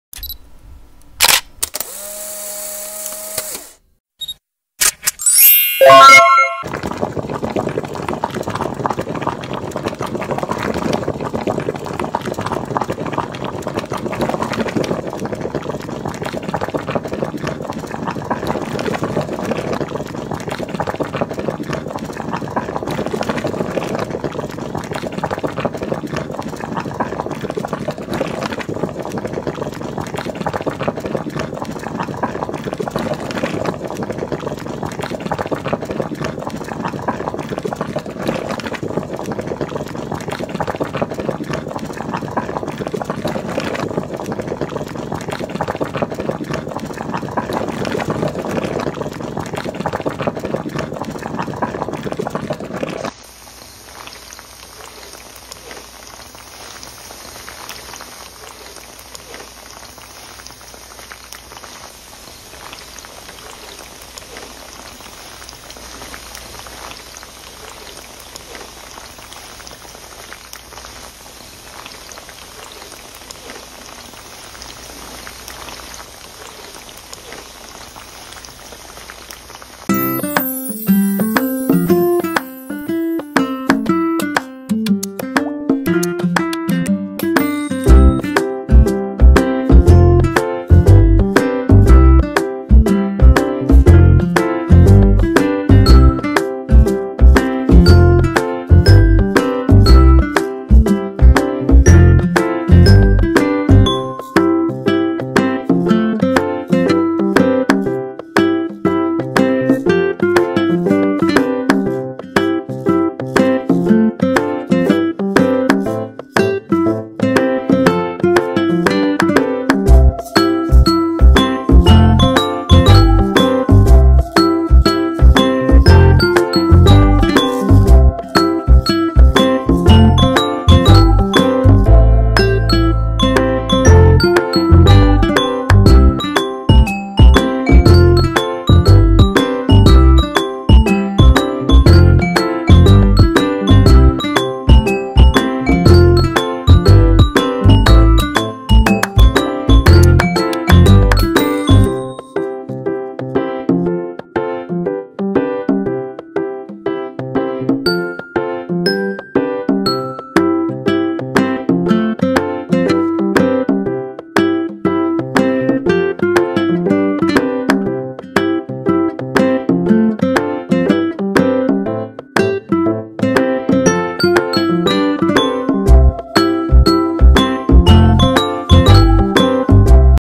BEEF BULALO+GRILLED SPANISH MACKEREL+FRIED TOFU+BAKED PLANTAINBANANAS+FRESH COCONUT WATER❤️JOYFUL JH
❤️BE A BLESSING!  LOVE  LIFE  “ALL”WAYS❤️

TITLE : BEEF BULALO SOUP +  GRILLED SPANISH MACKEREL /TANIGUE + FRIED CRISPY  TOFU/TOKWA + BAKED PLANTAIN BANANAS/SAGING NA SABA + RICE + FRESH BUKO JUICE ❤️JOYFUL JH❤️